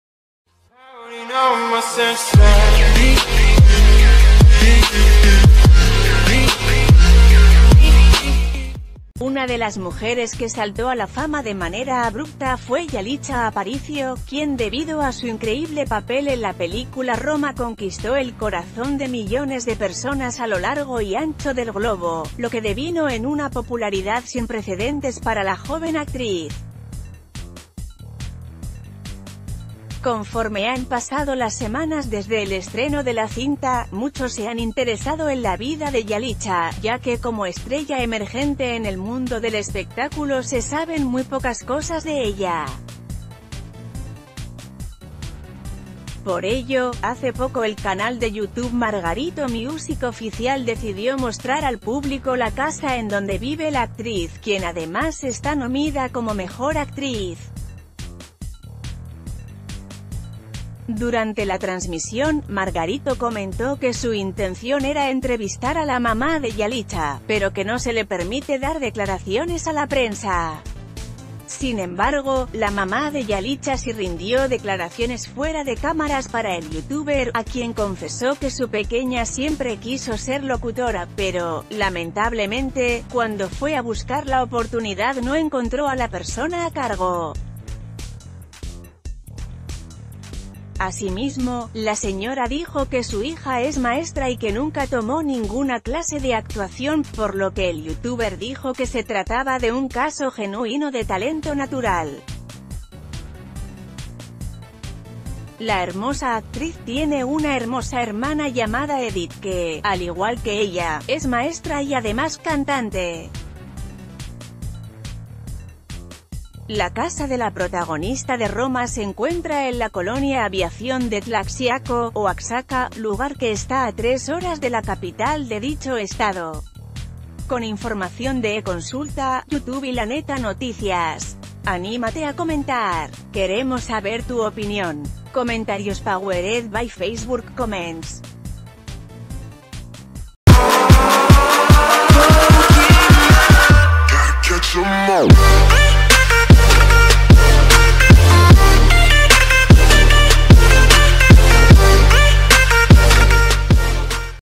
Aquí inició el sueño: mira la casa en que Yalitza Aparicio creció para triunfar
Una de las mujeres que saltó a la fama de manera abrupta fue Yalitza Aparicio, quien debido a su increíble papel en la película Roma conquistó el corazón de millones de personas a lo largo y ancho del globo, lo que devino en una popularidad sin precedentes para la joven actriz. Conforme han pasado l...